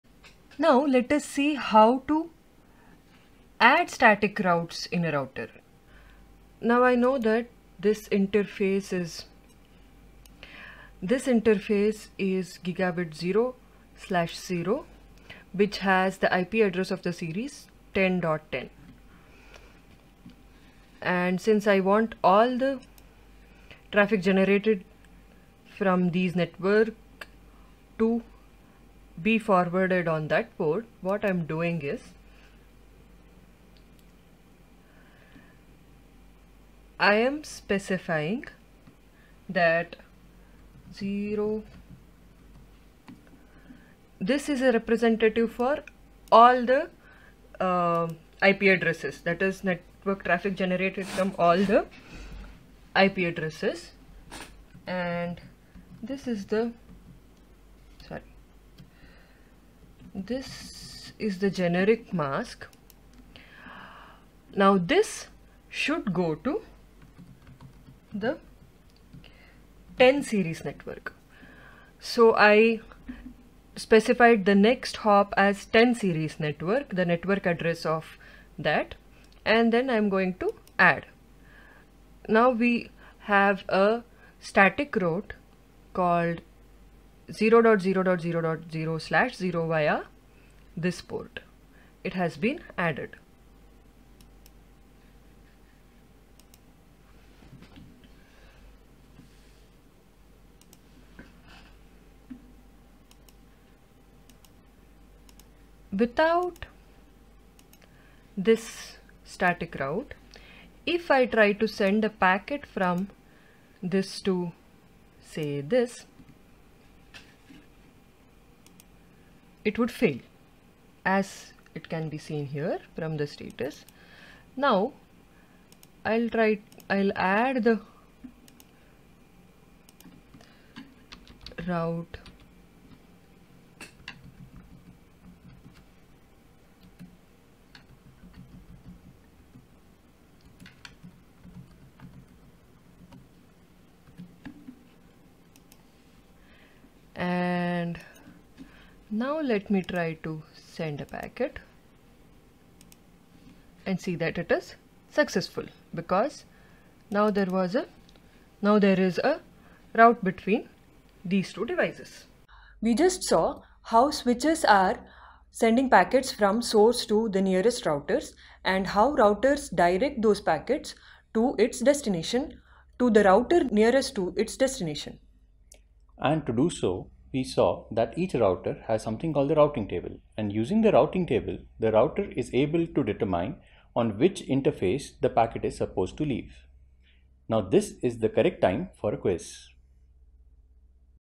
Week 2 Lesson 3.3 How to add static route in a router? (CPT)
Here we look at how to add routes in a router in Cisco packet tracer.